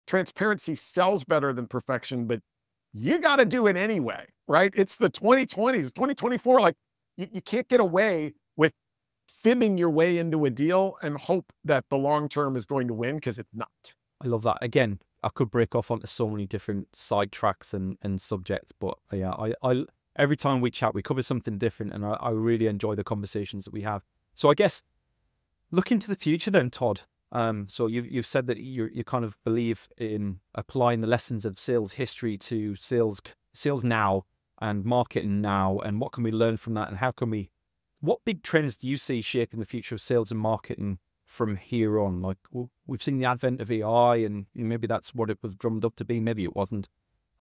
Transparency Sells Better Than Perfection 🌟
Attention all marketers and business owners! 📢

Don't miss our upcoming episode of MarketPulse: Pros and Pioneers on July 17th at 3 PM UK time! We’re thrilled to have Todd Caponi, the transparency sales expert and author of The Transparency Sale, join us to share his invaluable insights.

In this episode, Todd discusses:

The power of transparency in sales and marketing.
Why understanding deal losses can transform your strategy.
The risks of overusing AI in customer interactions.
Get ready to take your marketing to the next level.